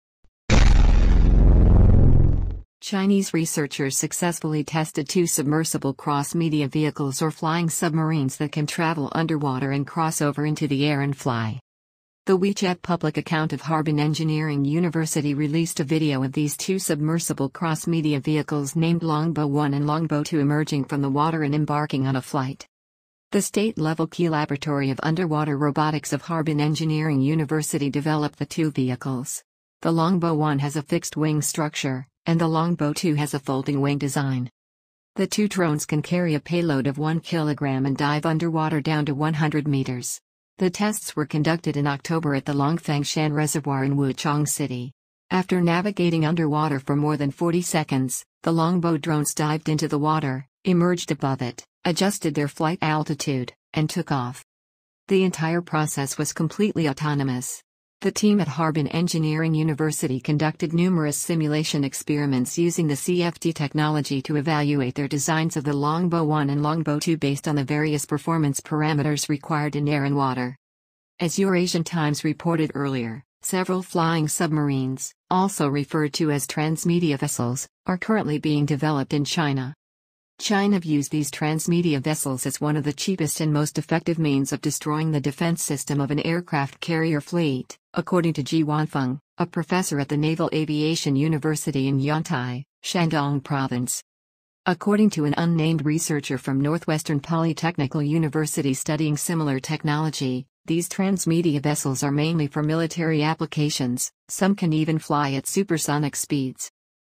China has test two new flying submarine that effectively to destroy defense of aircraft carrier
Chinese researchers successfully tested two submersible cross-media vehicles or ‘flying submarines’ that can travel underwater and cross over into the air and fly. Source